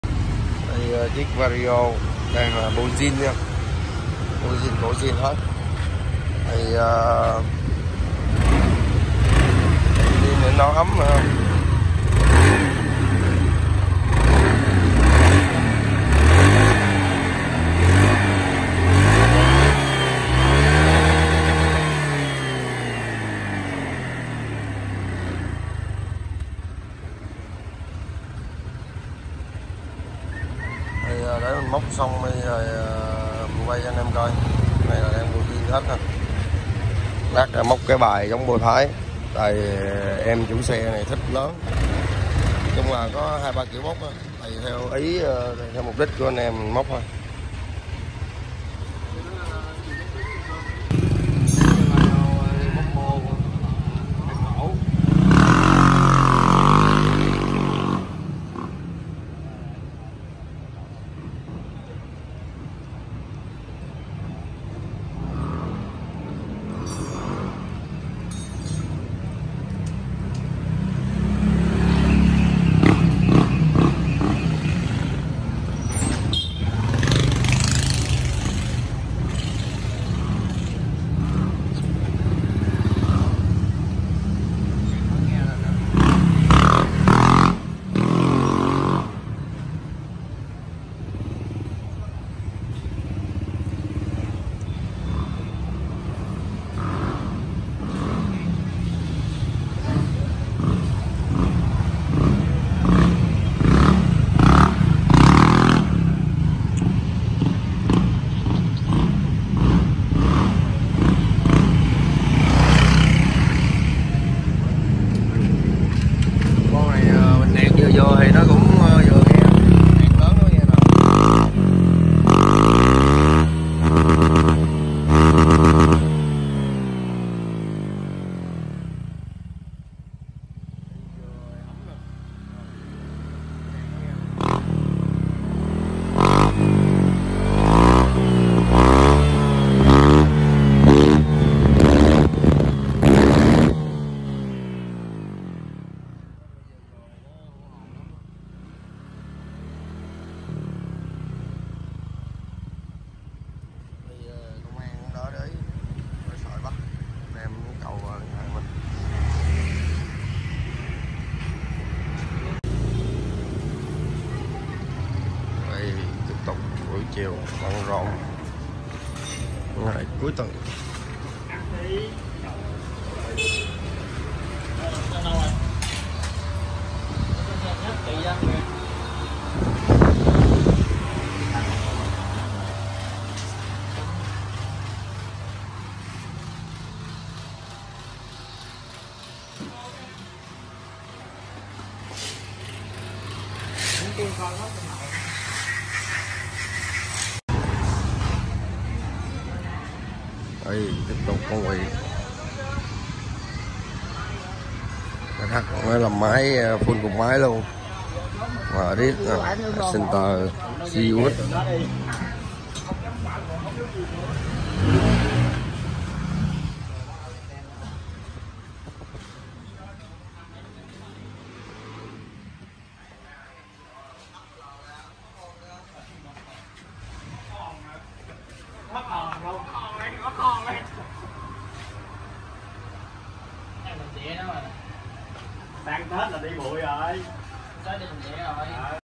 Honda Vario móc pô tăng tốc nẹt nghe khá hay nhưng đi chậm vẫn êm né công an bắt nha Ae tại TBK Team
Honda Vario móc pô tăng tốc nẹt nghe khá hay nhưng đi chậm vẫn êm né công an bắt nha Ae tại TBK Team 147 Tân Khai P4 Q11 Bảo Hello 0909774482